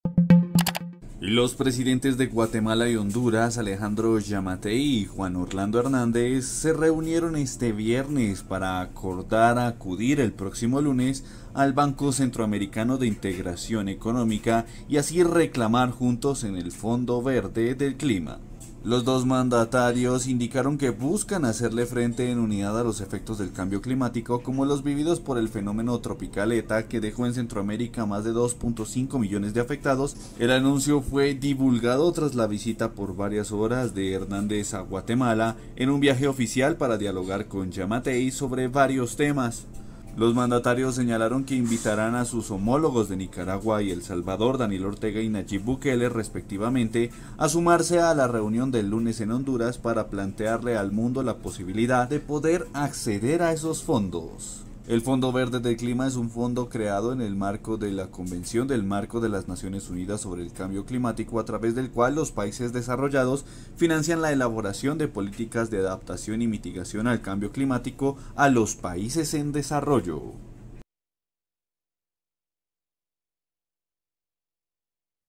Guatemala y Honduras se unen para pedir juntos el Fondo Verde del Clima
Durante una declaración conjunta en la Casa Presidencial guatemalteca, ambos mandatarios indicaron que buscan hacerle frente en unidad a los efectos del cambio climático, como los vividos por el fenómeno tropical Eta, que dejó en Centroamérica más de 2,5 millones de afectados y al menos 261 muertos en noviembre.

¡Visítenos!